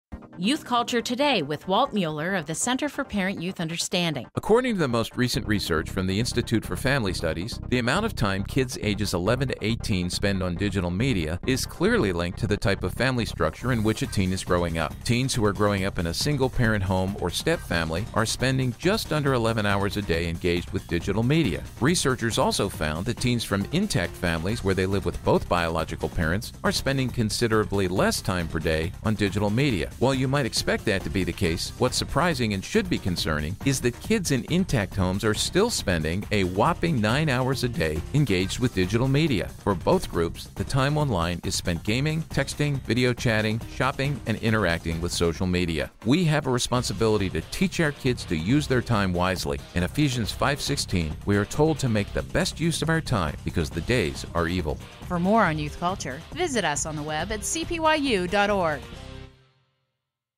Setting Screentime Limits
According to the most recent research from the Institute for Family Studies, the amount of time kids ages eleven to eighteen spend on digital media is clearly linked to the type of family structure in which a teen is growing up. Teens who are growing up in a single-parent home or step-family are spending just under eleven hours a day engaged with digital media. Researchers also found that teens from intact families where they live with both biological parents, are spending considerably less time per day on digital media. While you might expect that to be the case, what’s surprising and should be concerning is that kids in intact homes are still spending a whopping nine hours a day engaged with digital media. For both groups, the time online is spent gaming, texting, video chatting, shopping, and interacting with social media. Parents, we have a responsibility to teach our kids to use their time wisely. In Ephesians five sixteen we are told to make the best use of our time, because the days are evil.